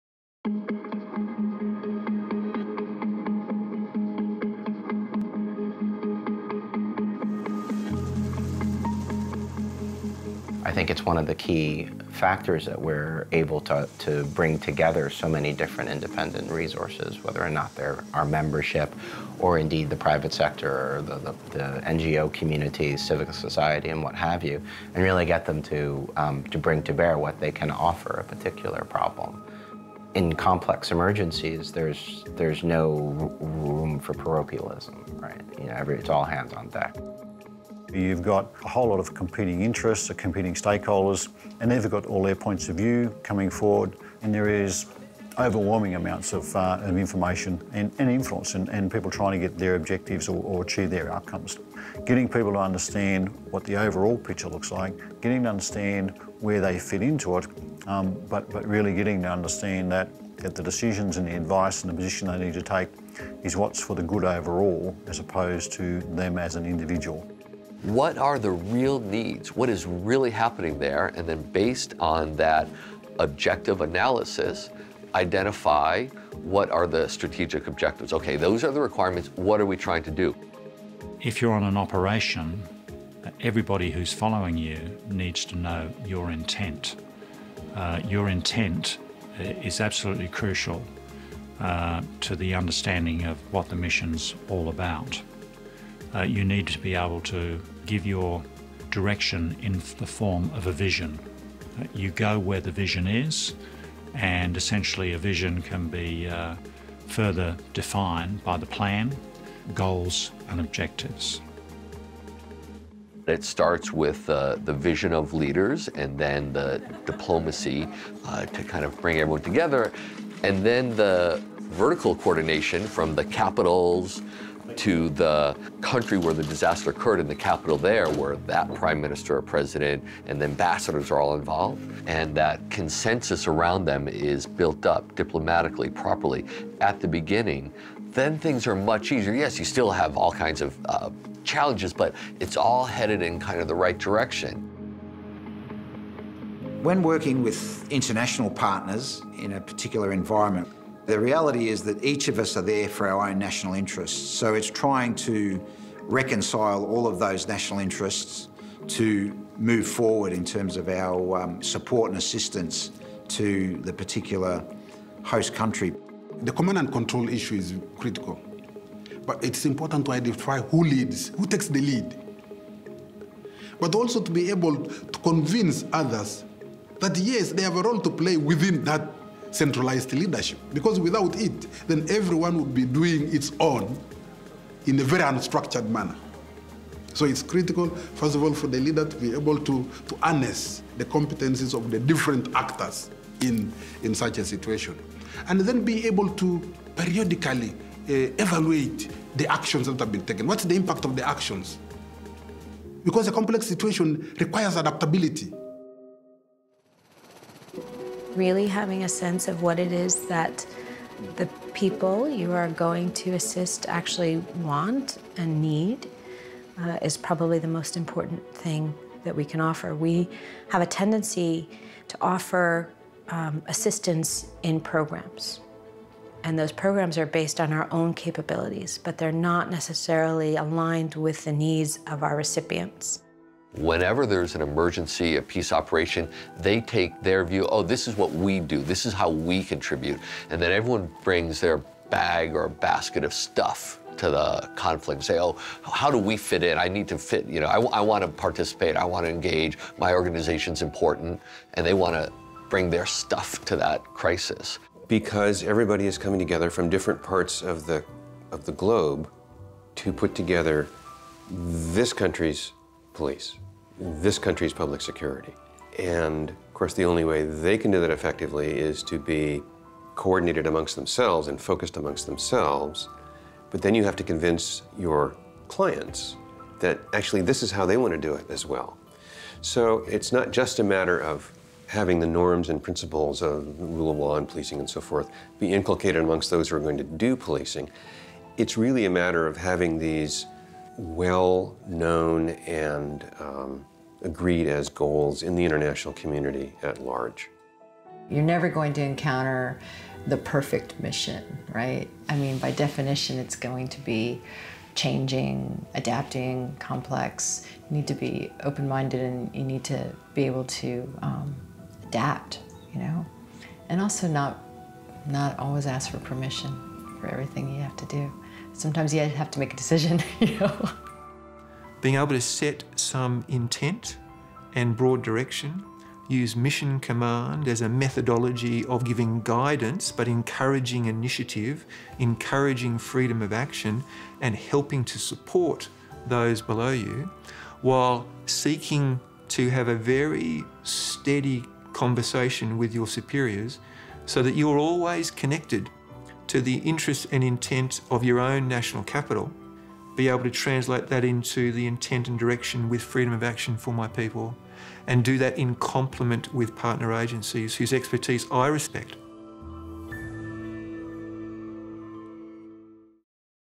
Leading Together Chapter 2 - Strategic Objectives
How does each organisation fit into the broader picture
to meet the needs of the communities being served?

Leaders examine the process of developing mission objectives across a whole-of-mission framework, highlighting that each stakeholder has a role to play in defining and meeting the strategic goals of any response effort.